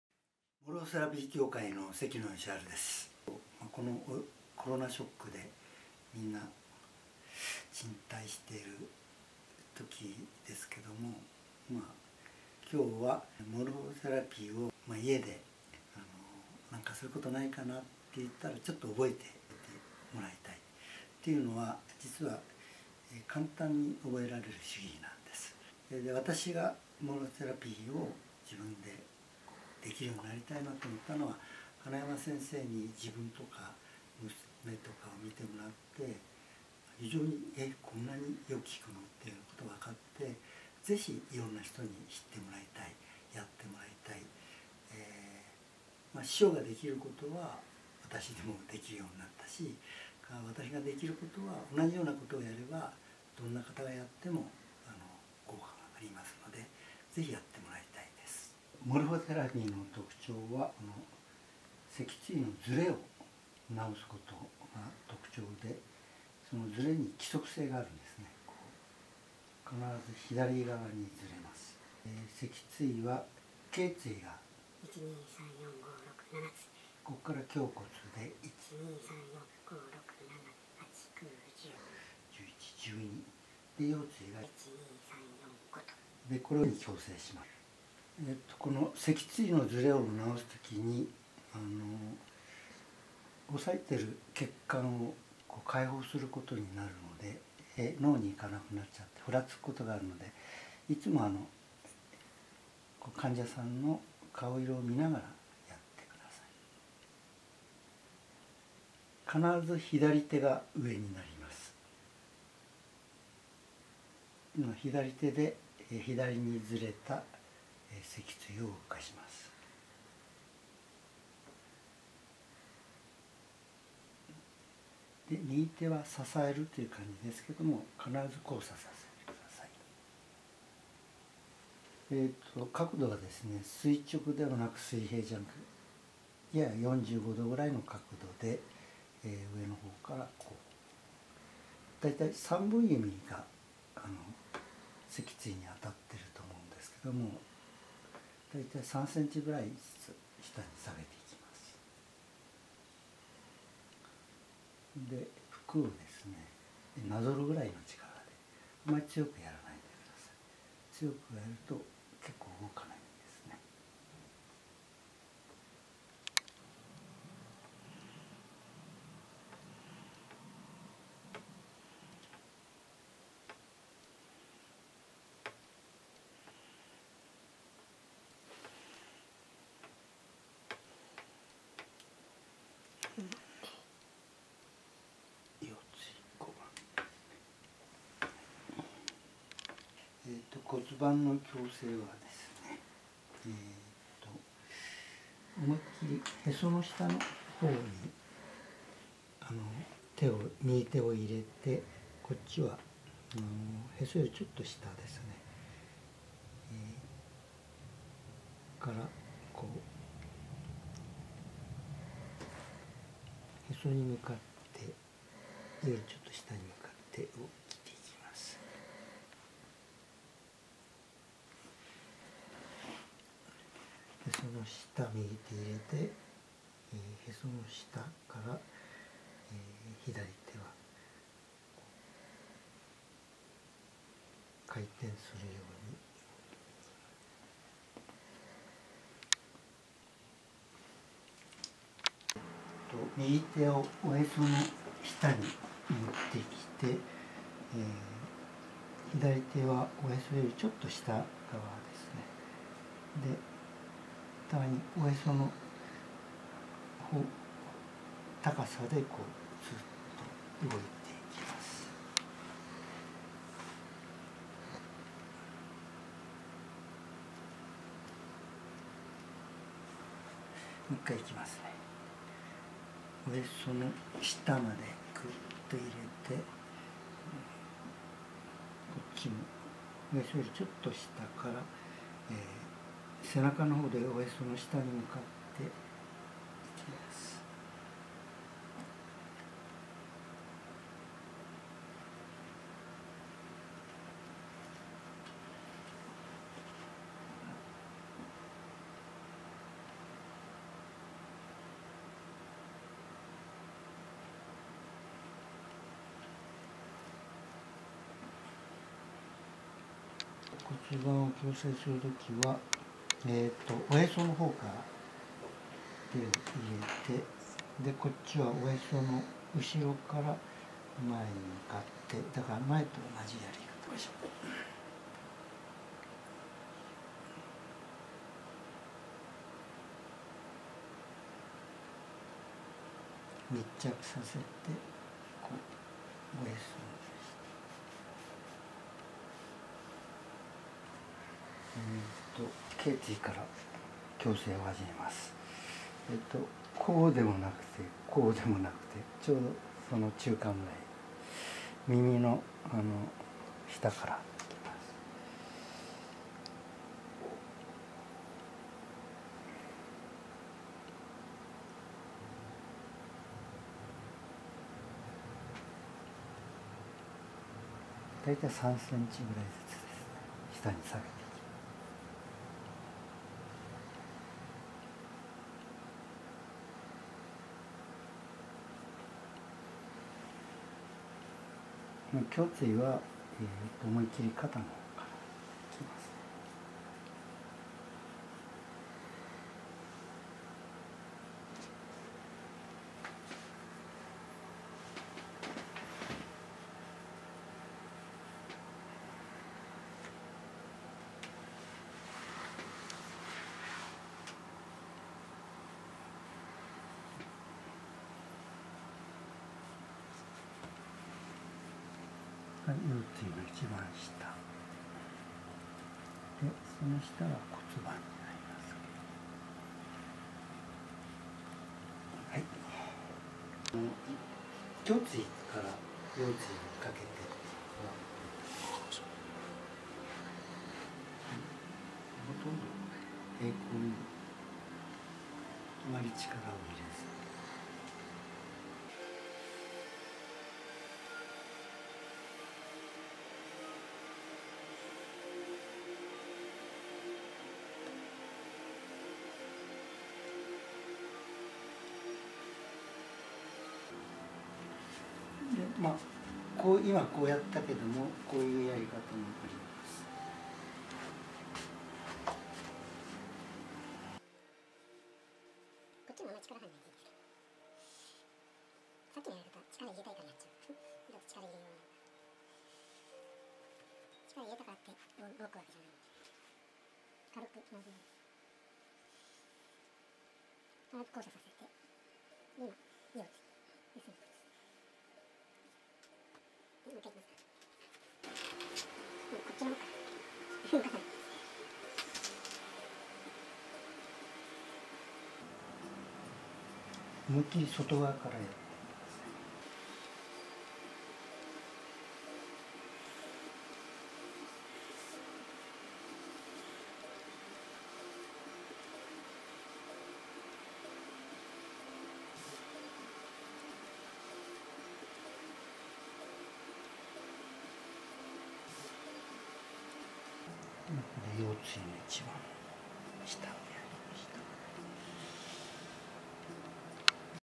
골격 교정 모르포테라피（Morphotherapy）초급편
신형 코로나 바이러스로 인한 행동 자제 요청이라는 보도를 듣고 제가 도움이 될 일을 생각하다가 급히,
 제가 의사의 입장에서 실시하는 모르포세러피를 알리는 동영상을 만들었습니다.
그레이트 쟈니를 포함해서 50년 가까이 전 세계를 여행하면서 많은 사람에게 신세를 졌습니다.
코로나 바이러스로 마음이 풀리지 않는 상태가 계속 될지도 모릅니다만, 도움이 된다면 다행입니다.
세키노 요시하루(関野吉晴)

하나야마 수이세이(花山水清) (일본어 페이지)

모르포테라피협회 (일본어 페이지)

기술협력위원 모리 류이치(森龍一)의 기사
“크레용 하우스　 "iine(좋아요)" vol.47 뼈에 대해 생각해 보는 게 좋겠어!"

※링크지는 일본어 HP

新型コロナでの行動自粛要請という報道を聞き、私で役立てる事を考えた所、急遽、私が医師の立場で行なっているモルフォセラピーの事を伝える動画を作りました。
 グレートジャーニー含めて50年近く世界中を旅してきて沢山の人のお世話になりました。
 コロナで気が晴れない状態が続いてしまうかも知れませんが、お役に立てたら幸いです。関野吉晴

花山水清 (日本語ページ)

モルフォセラピー協会 (日本語ページ)

 技術協力委員　森龍一の記事　「クレヨンハウス　 「いいね」vol.４７　骨について考えたほうがいい！ 」

リンク先は日本語HP

※음성과 자막이 다른 곳이 있습니다　音声と字幕が違う所があります
자막 일본어　字幕 日本語
地球永住計画代表モルフォセラピー協会医療顧問の関野吉晴です
「コロナショックで家にいるときに(親しい人同士で) 出来るモルフォセ ラピーを紹介します。」実は簡単に覚えられる手技なのです。私がモルフォセラピーを自分で出来るようになりたいと思ったのは、 創 始者の花山水清氏に
(様々な部位の痛みや姿勢不良などに) 自分や娘や友人を見てもらって効 果を実感したからです。こんなときだからこそ、まずは知って下さい。 そして試して欲しいと 思います。花山氏が提唱するモルフォセラピーの初級編を行う事で 私も多くの人に効 果を実感して貰える様になりました。
 私が出来る事を同じ様に行えば 誰が行っても(思った以上の)効果が出る可能性があるので ぜひ親しい人同 士で試してみてください。
《モルフォセラピーの特徴》花山水清 廣済堂出版 で検索 概要欄にリンクあり　脊椎のズレを戻すことです。脊椎のズレには規則性があります。必ず左側にズレます。
頚椎、つまり上から順に矯正をしていきます。
モルフォセラピーをおこなう前に症状の確認や体の動きを確認してから 始めて下さい

「背骨のズレを直すときに 抑えられてる血管を開放するためか まれに目眩のような状態になる場合があるので ふらつくことがあります。」会話したり、
相手の顔色や状況を見ながらやって下さい。・必ず左手を上にします。・左手で左にズレた脊椎を 動かすイメージで手を滑らせます。・右手は下の骨を支えるイメージですが、
途中で手を止めずに真ん中で交差させる。左手の角度は、右上がりのななめ45度 位の角度で右手は逆です。
上の方から行います。
三本指 (人差し指、中指、薬指) で 背中の両端から真ん中に向かって 背骨を超えて手を交差させます。 3センチ位ずつ下に下げていきます。服をなぞるくらいの力で、 強くならないように気をつけてください。 ・むしろ強いと上手くいきません。 再生速度×2 ・会話をしながら、お互いが疲れない程度に 何往復か繰り返しおこなって下さい。座った状態で椅子や床まで 手が届くまでおこなってOKです。
骨盤の矯正
骨盤の矯正はへその裏に右手を入れて左手はへその斜め下から骨盤の上の縁(腸骨稜)をさするように 左に滑らせます。 手の動きは逆になります。(右手は身体の背部から腹部に、 左手は背中へと手を滑らせます)(右手は身体の背部から腹部に、左手は背中へと手を滑らせます)
実技編
おへその後に右手を入れて 左手はへその下の方から 骨盤の上の端(腸骨稜)をさするように 左に滑らせます. 右手は身体の背部から腹部に 左手は背中へと手を滑らせます。 手の動きは逆になります
※力を入れずに手を密着させて滑らせて下さい。※疲れない程度に何回か 繰り返し行なって下さい。骨盤を後ろから矯正 施術者からは仰向けで行うのと 手の動きは同じです。座位でも同じ流れです。 座位でも試して下さい
《座位でおこなう背骨の矯正》
・手の角度は斜め45度で行います。・頚椎から順におこなうので、 左手は耳たぶのあたりに 右手は耳たぶの下に手を置いて、 手が交差出来るように力を抜いて 3センチずつ下げていきます。途中で身体変化のチェックもしてください
※体の横(外側)から始める *できるだけ手の動きを大きく、ゆっくりとおこないます。・胸椎は肩に手をそえてから、
背骨を超えて逆側に引っ張るような イメージでおこないます。受け手の方は、少しでも不快感を感じたら 施術者に声をかけて休んで下さい。※右手が椅子や床につくまで 複数回おこなって下さい。
【手の使い方の応用】
施術者が疲れない様に楽に出来る立ち位置を探して下さい。※手を逆手(クロス)にしたり、受け手の右側から行う等の試行錯誤も試して下さい 再生速度 x2※ 両方行ってもOKです。最初は触るだけの弱い力でゆっくりとおこない、慣れてきたら速さ、角度を調整してみて下さい。※シンプルなので工夫しておこなってみて下さい。
以上、モルフォセラピー初級編をお伝えしました。 さらに知りたい方は 協会が行う講座もありますので 概要欄のリンクをご確認下さい。
概要欄にリンクを貼ります。皆様くれぐれも元気にお過ごし下さい。医師 探検家 グレートジャーニー 関野吉晴 編集、制作 森龍一